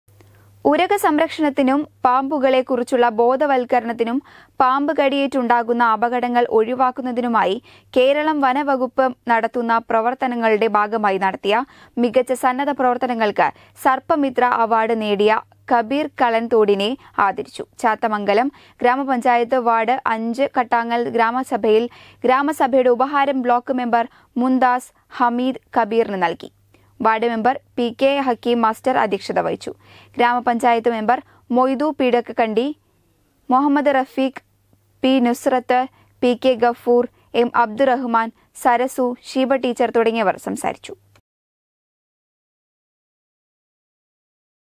മികച്ച സന്നദ്ധ പ്രവർത്തനങ്ങൾക്ക് സർപ്പ മിത്ര അവാർഡ് നേടിയ കബീർ കളൻതോടിനെ ആദരിച്ചു.
ഉരഗ സംരക്ഷണത്തിനും പാമ്പുകളെ കുറിച്ചുള്ള ബോധവൽക്കരണത്തിനും പാമ്പ് കടിയേറ്റുണ്ടാകുന്ന അപകടങ്ങൾ ഒഴിവാക്കുന്നതിനുമായി കേരളം വനം വകുപ്പ് നടത്തുന്ന പ്രവർത്തനങ്ങളുടെ ഭാഗമായി നടത്തിയ മികച്ച സന്നദ്ധ പ്രവർത്തനങ്ങൾക്ക് സർപ്പ മിത്ര അവാർഡ് നേടിയ കബീർ കളൻതോടിനെ ആദരിച്ചു.